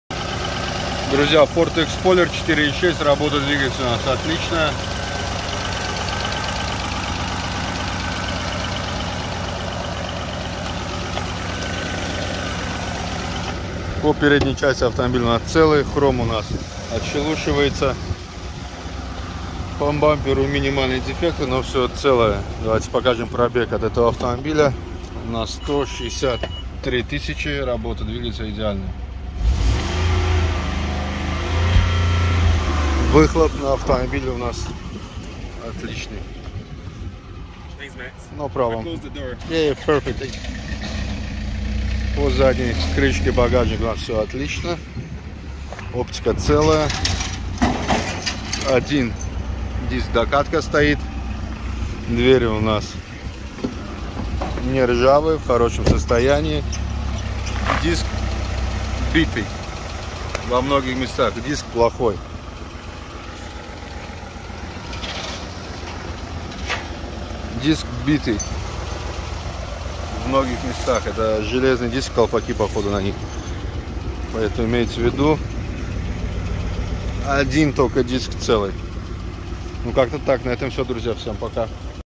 Разборка Ford - Explorer 2006-2010, 4.6 (VIN: 1FMEU75857UA71095)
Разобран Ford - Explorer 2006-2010
Основные характеристики:
Год выпуска: 2007
Двигатель:
Объём двигателя: 4.6
Тип двигателя: Бензин
Коробка передач: КПП автомат 4х4 (АКПП)
Кузов: Джип (5-дверный)
Смотри каталог запчастей Ford - Explorer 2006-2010 на :
⏰ Режим работы колл-центра:
08:00 – 20:00 – ежедневно

Для наших покупателей:
- бесплатная доставка;